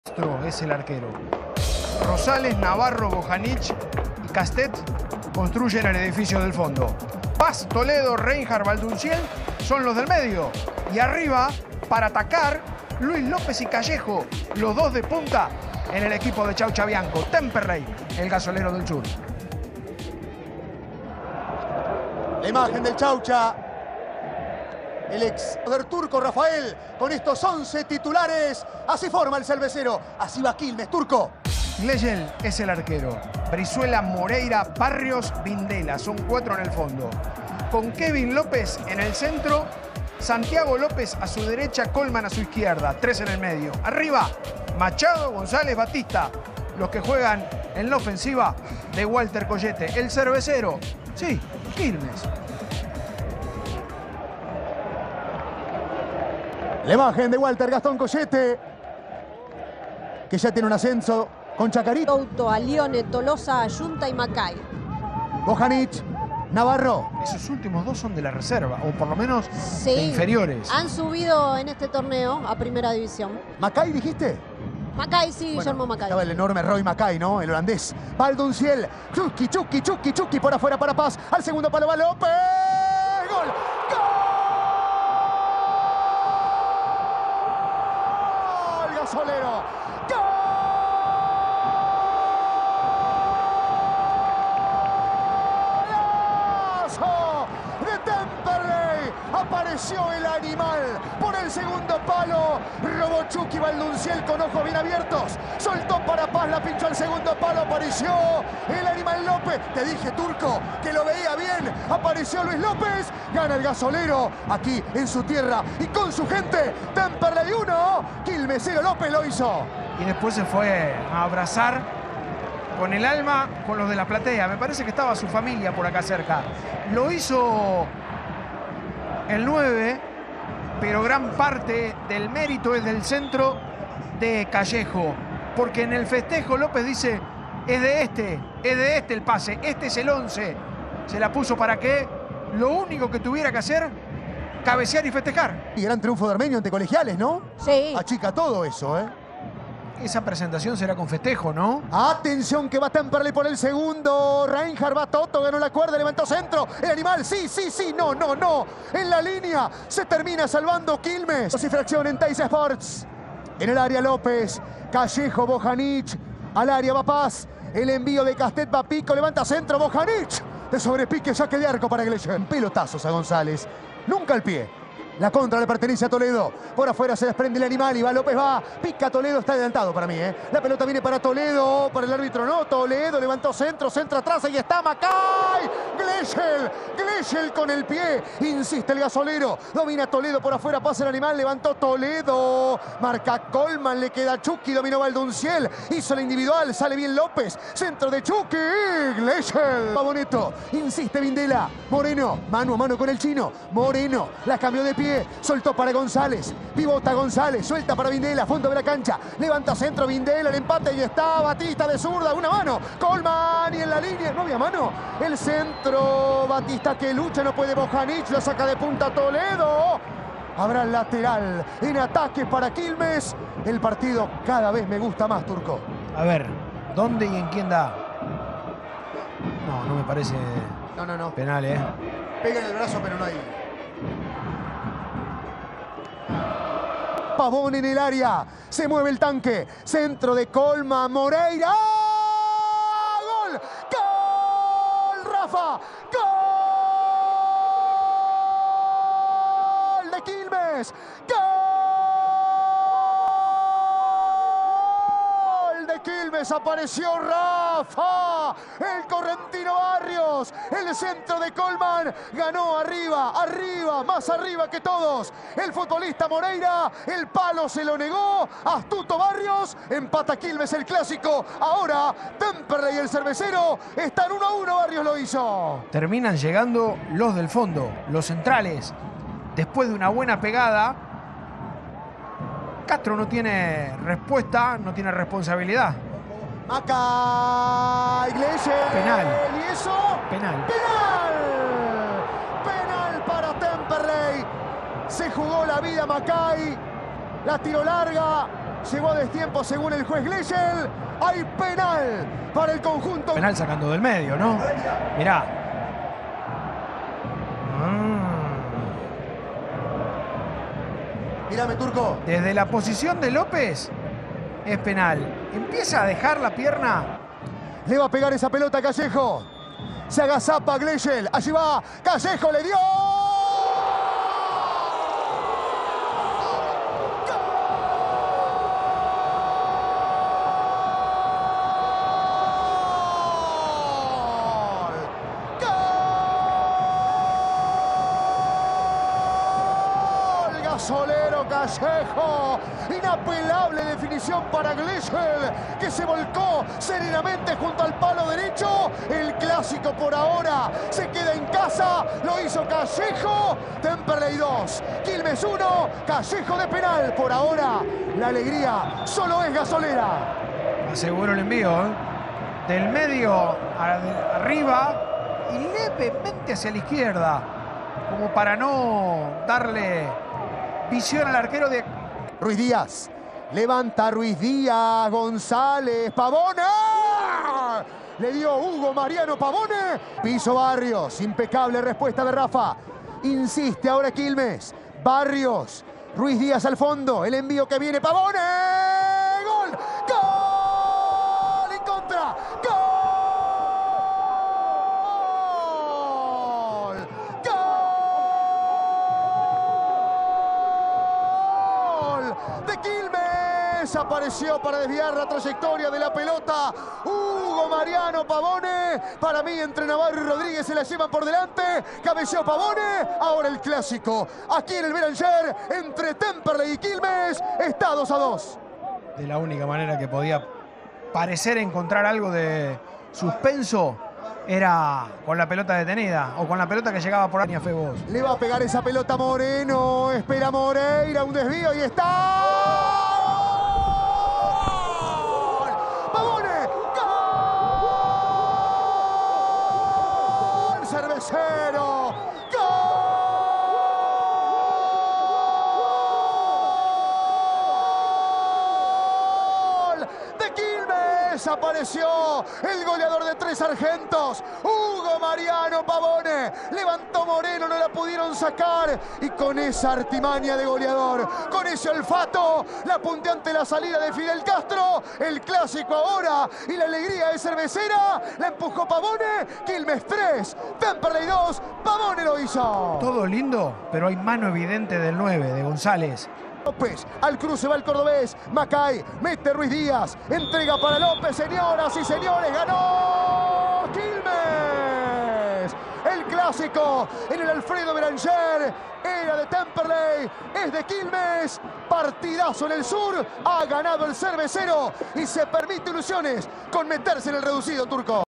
Temperley 2-3 Quilmes | Primera Nacional | Fecha 30
Goles de Luis López (TEM) a los 28', de Rafael Barrios (QAC) a los 77', de Facundo Callejo (TEM) a los 80', de Ezequiel Navarro -e/c- (QAC) a los 90(+2)' y de Mariano Pavone (QAC) a los 90)+4)'.Seguí toda la Primera Nacional en TyC Sports y TyC Sports Play. Lo mejor del deporte está en TyC Sports. Youtube Facebook ​ Twitter ​ Instagram ​